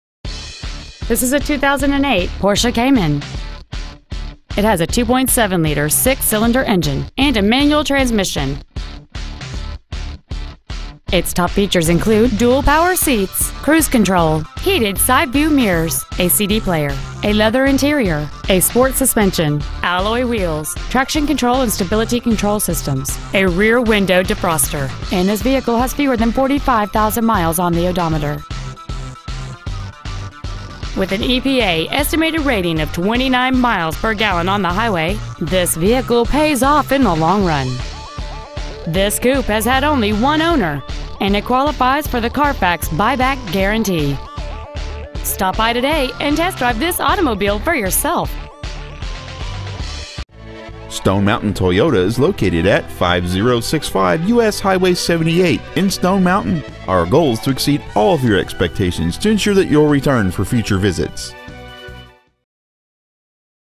2008 Porsche Cayman Stone Mountain GA 30087
We are proud to present this 2008 Porsche Cayman .

 Year : 2008
 Make : Porsche
 Model : Cayman
 Engine : 2.7L H-6 cyl
 Trans . : Manual
 Exterior : Basalt Black Metallic
 Miles : 44,273
 Interior : Black Leather
 5065 U.S. Highway 78
 Stone Mountain , GA 30087
 Preowned 2008 Porsche Cayman Stone Mountain GA